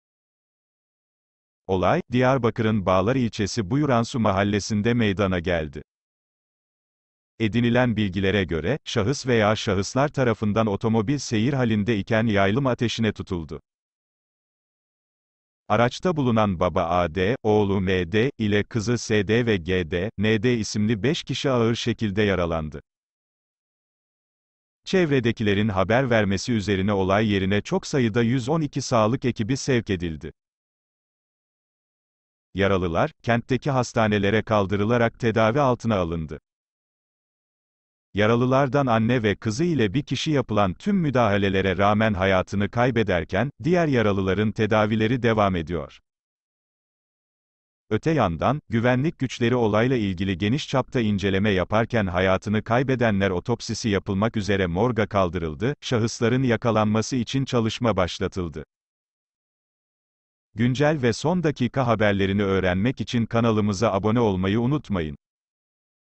Diyarbakır’da araç tarandı!
Diyarbakır’da araç tarandı! Ölüler ve yaralılar var

Diyarbakır’da akşam saatlerinde otomobil tarandı. Otomobilde bulunan 5 kişiden 3’ü hayatını kaybettiği belirlendi.

Olay, Diyarbakır'ın Bağlar ilçesi Buyuransu Mahallesinde meydana geldi. Edinilen bilgilere göre, şahıs veya şahıslar tarafından 33 DUG 48 plakalı otomobil seyir halinde iken yaylım ateşine tutuldu.

5 KİŞİ AĞIR YARALANDI

Araçta bulunan baba A.D, oğlu M.D, ile kızı S.D ve G.D, N.D isimli 5 kişi ağır şekilde yaralandı. Çevredekilerin haber vermesi üzerine olay yerine çok sayıda 112 sağlık ekibi sevk edildi.

3 KİŞİ HAYATINI KAYBETTİ

Yaralılar, kentteki hastanelere kaldırılarak tedavi altına alındı. Yaralılardan anne ve kızı ile bir kişi yapılan tüm müdahalelere rağmen hayatını kaybederken, diğer yaralıların tedavileri devam ediyor.

Öte yandan, güvenlik güçleri olayla ilgili geniş çapta inceleme yaparken hayatını kaybedenler otopsisi yapılmak üzere morga kaldırıldı. Şahısların yakalanması için çalışma başlatıldı